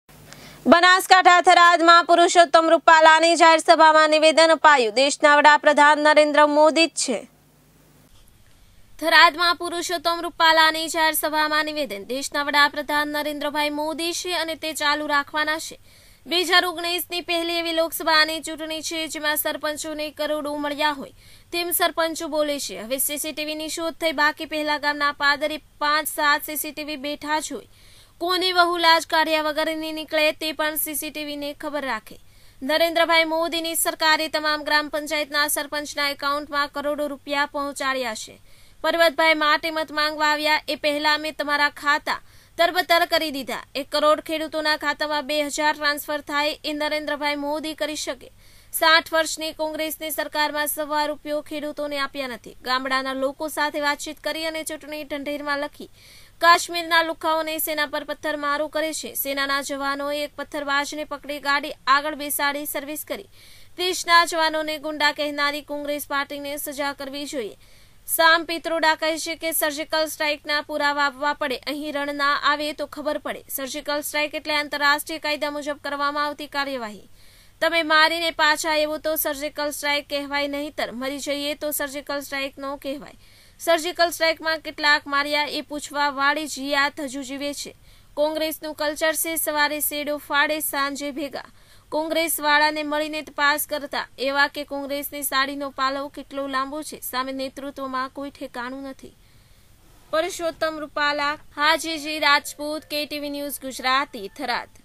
બનાસકાંઠા..... થરાદ માં પુરુષોત્તમ રૂપાલા ની જાહેર સભા માં નિવેદન. આપ્યું .....દેશના
બનાસકાંઠા. થરાદ માં પુરુષોત્તમ રૂપાલા ની જાહેર સભા માં નિવેદન. આપ્યું .દેશના વડાપ્રધાન નરેન્દ્રભાઈ મોદી છે